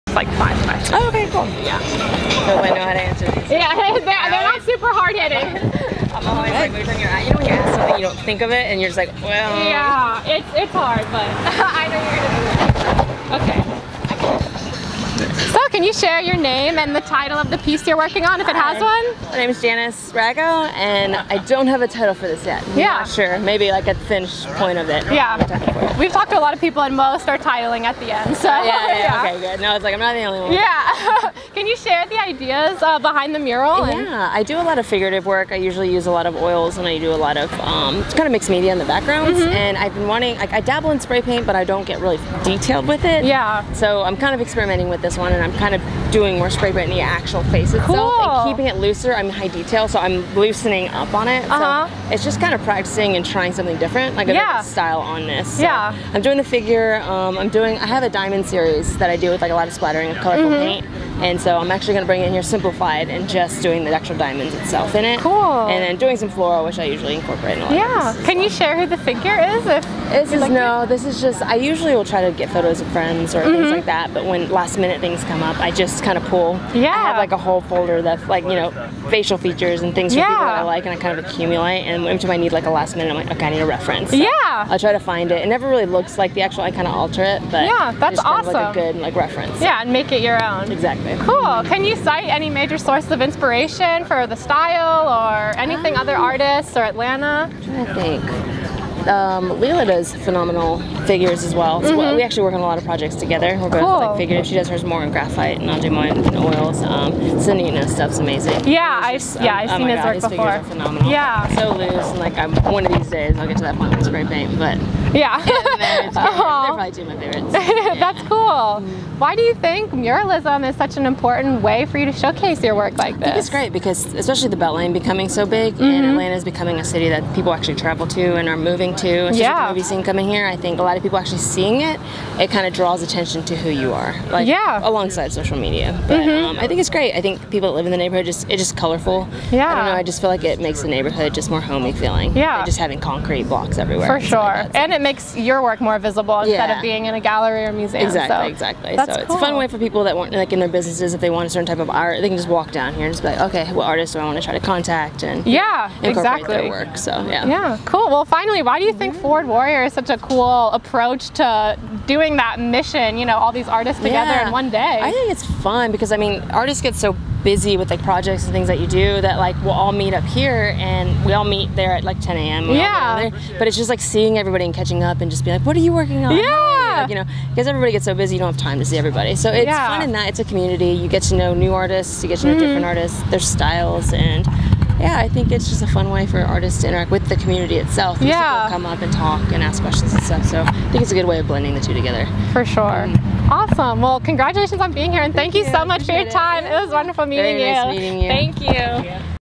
Artist Interview : Janice Rago - Forward, Warrior!! 2018
Artist Janice Rago interviewed by Claire Moore during "Forward, Warrior!!" 2018 on Wylie Street in Cabbagetown USA.

Over 40 artists participate yearly in Atlanta's best street art event, adding exciting new work to a half-mile art installation along the CSX Wall. For over 10 years, the Cabbagetown Initiative 501c3 & Cabbagetown Neighborhood Improvement Association have cooperated with local curators to promote free speech and social equity, while beautifying the urban landscape in the Cabbagetown Landmark Historic District.

"Forward, Warrior!!" was created by Georgia artist Peter Ferrari (Facet Gallery) as an inspirational happening for an inclusive collective of established & emerging artists. From Carroll Street to Pearl Street, hundreds of murals have been created, intertwined, merged, morphed, and evolved into the largest & longest continuous collaborative art piece in the Southeast.

Facebook.com/ForwardWarrior
Instagram.com/ForwardWarrior